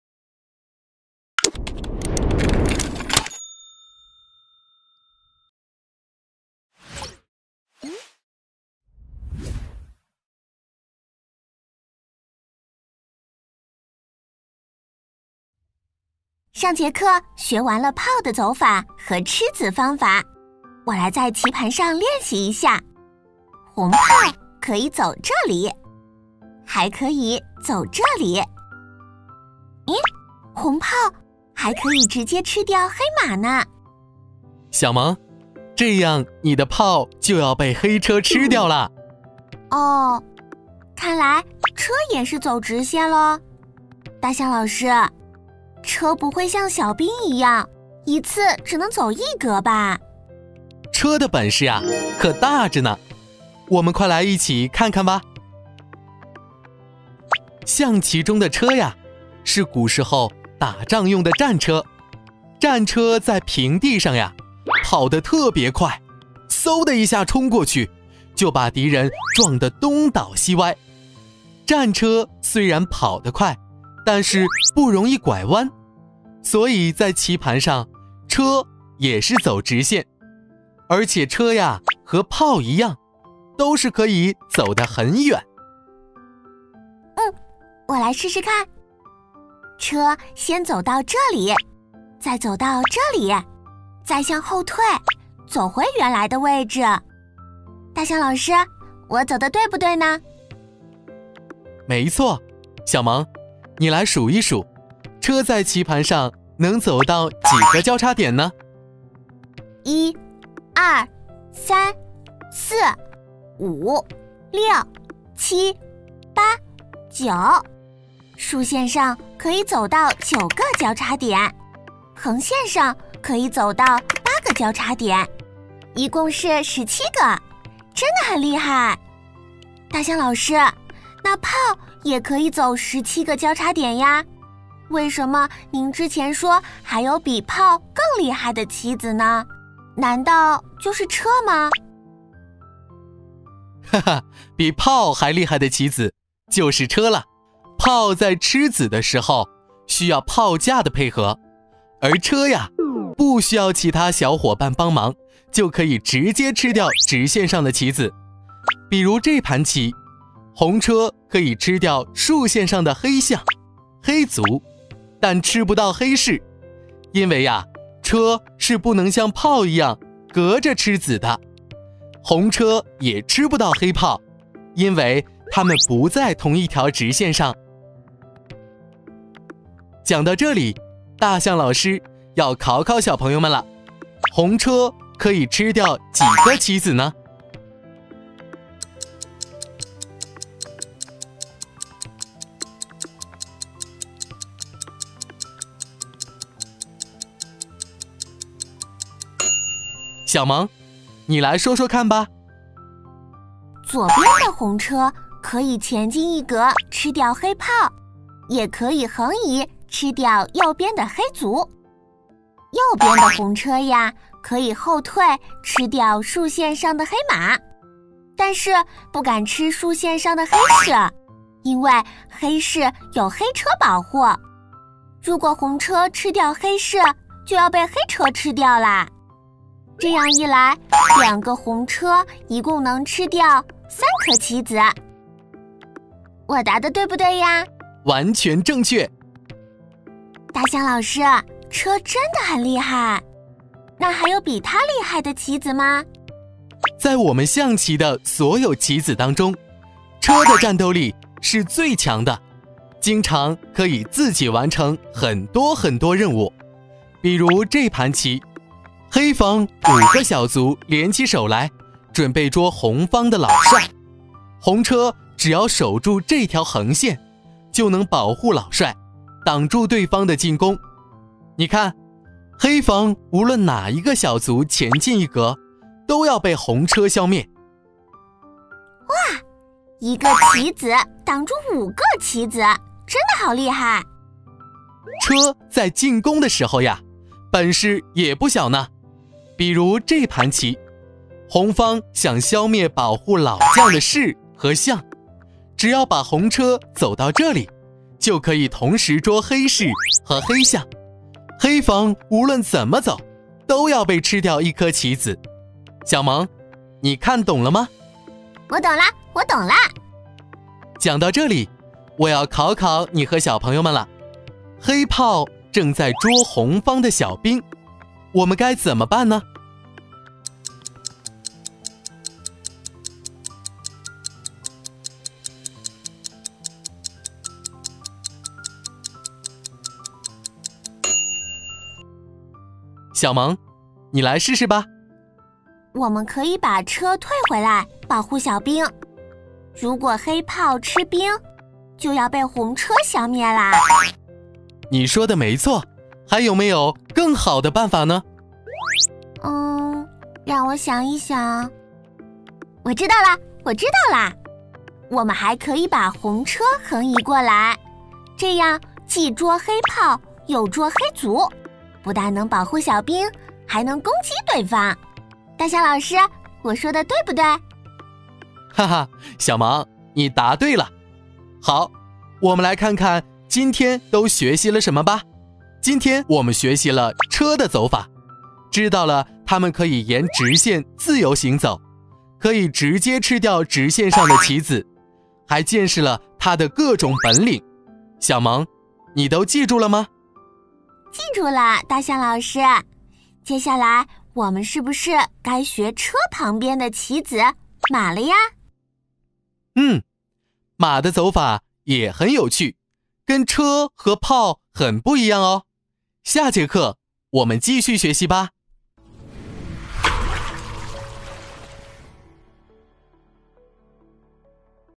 第5节：车的走法
了解车的走法，主要有两个重点：沿直线行棋，步数不限；吃子不需要配合，可以直接吃掉同一条直线上的棋子，但不能隔着吃子。进一步学习车的吃子和攻防中的运用。